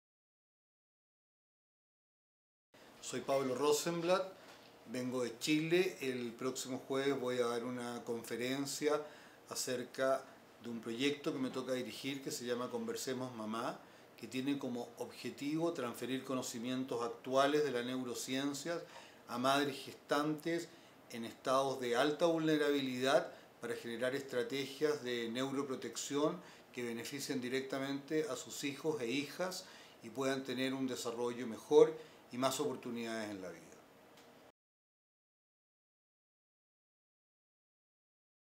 Pablo Rosenblatt / Conversemos mamá
Invitación a la conferencia:
Las neurociencias en acción.

Auditorio del CEIICH, Torre II de Humanidades 4º piso. Circuito Escolar, Ciudad Universitaria, Cd.Mx.

Coordinador(a): Aquiles Negrete Yankelevich

Participante(s): Pablo Rosenblatt/ Instituto Milenio de Oceanografía, Chile. Premio Internacional Tribute Award 2016

PAPIIT-IN301815